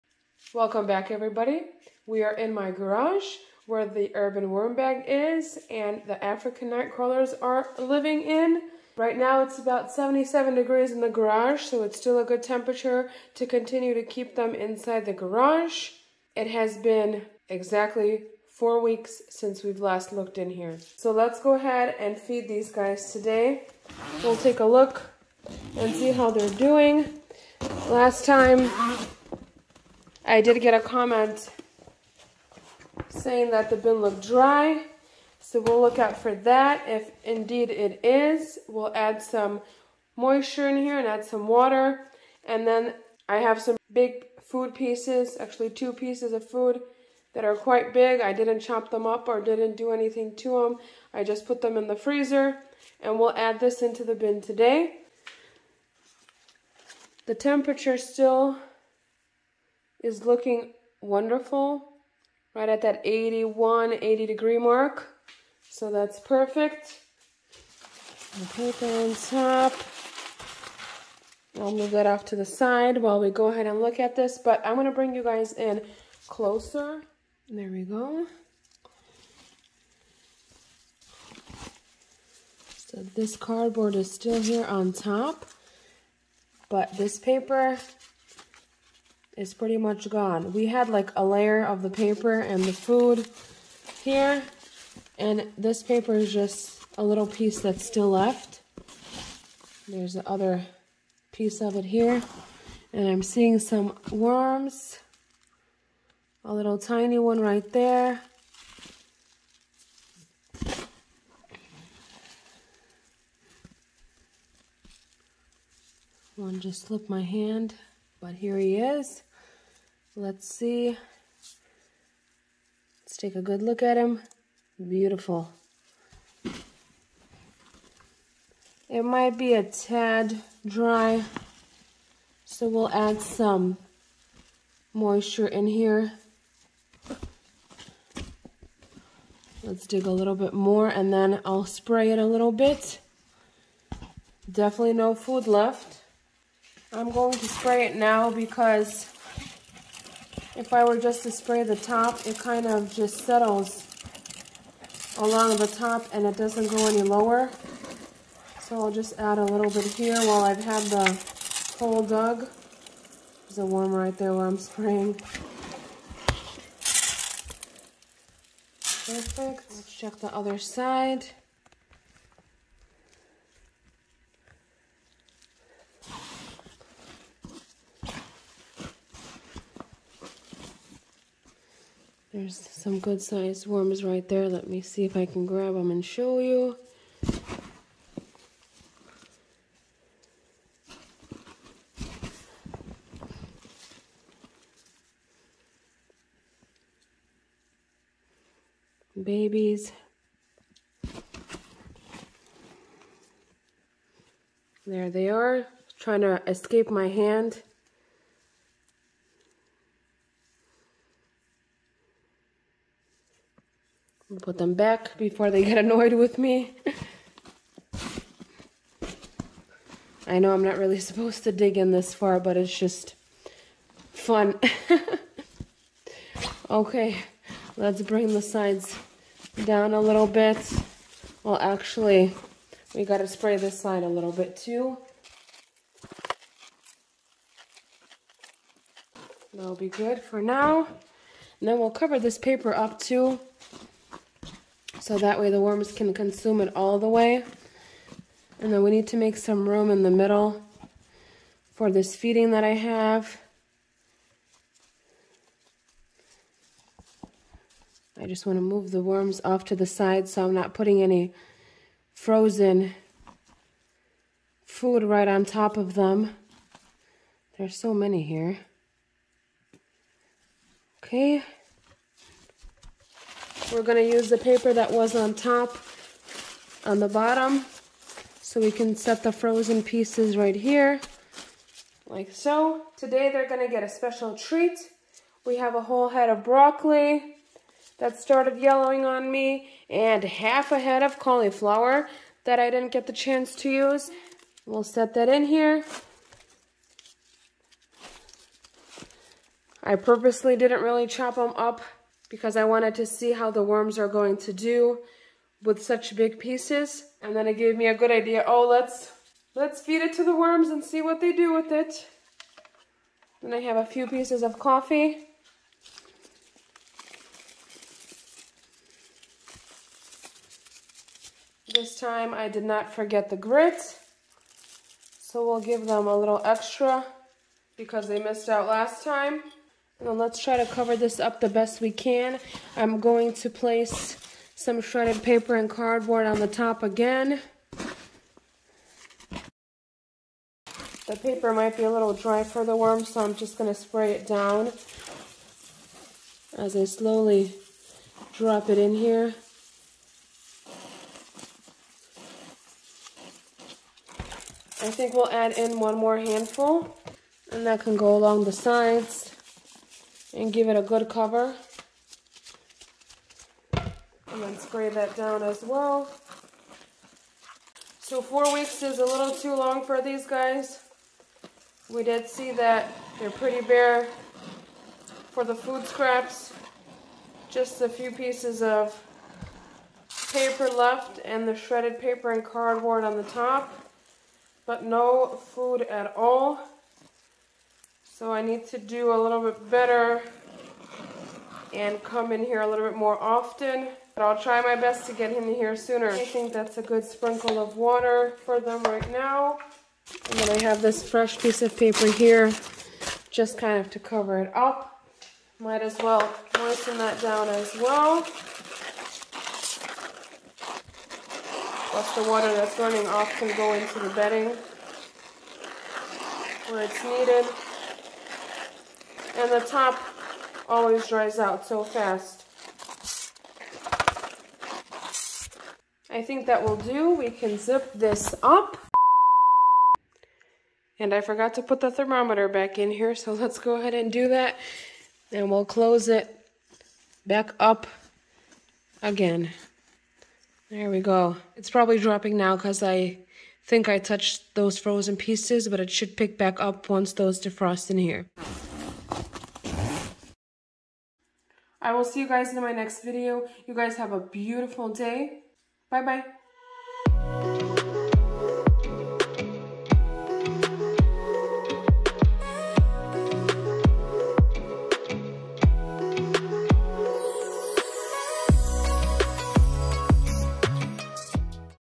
ANCs Fed LARGE Pieces Of Broccoli And Cauliflower | African Nightcrawlers | Urban Worm Bag
After another 4 weeks, the African Nightcrawlers that are in my Urban Worm Bag are in much need of a feeding!
Today they received 1 whole head of broccoli and half of a cauliflower head!
More shredded paper and carboard was provided as well!